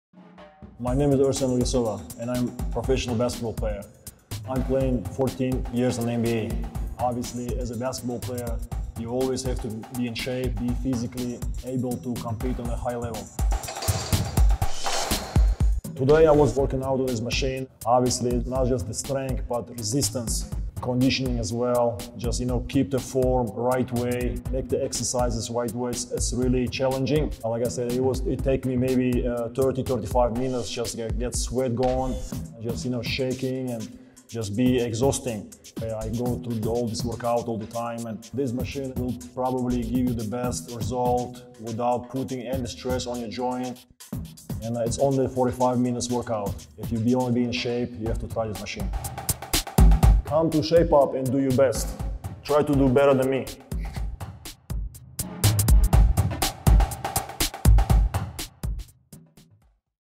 Try to do better than Ersan Ilyasova! (w/ English Subtitle)
Megaformer is a workout that makes everyone struggle, even NBA players like Ersan Ilyasova. No matter how strong, athletic or professional you are, you will find it very challenging. However, remember that where there is no struggle there is no improvement, so how you take that challenge is completely up to you!